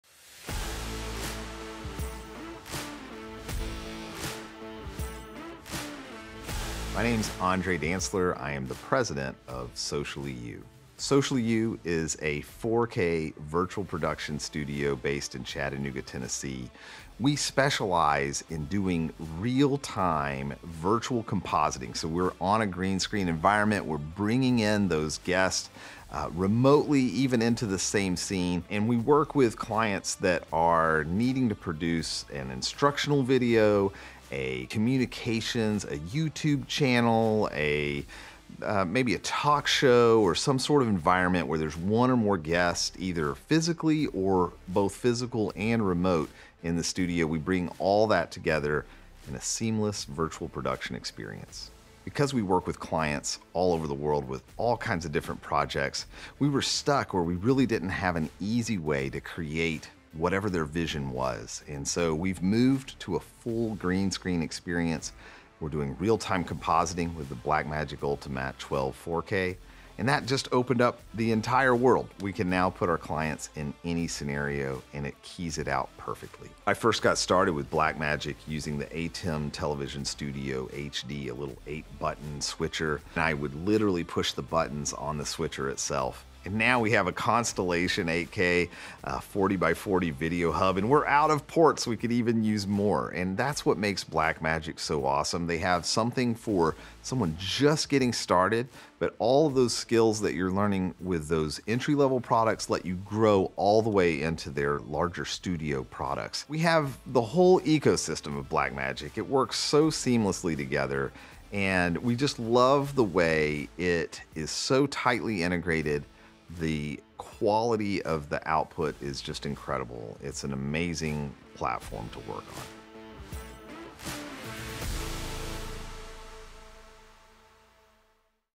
SociallyU Goes Virtual with Ultimatte 12 4K
Eight Ultimatte 12 4K real time compositing processors are being used to revolutionize virtual sets at SociallyU, a Chattanooga, TN based video production studio. SociallyU specializes in real time editing and virtual production, backed by an 18 ft. by 30 ft. greenscreen stage and an end to end Blackmagic Design workflow, including six Blackmagic Studio Camera 6K Pros. With a firm commitment to delivering exceptional videos at record speed, SociallyU operates by the slogan “We make videos as fast as you can talk.”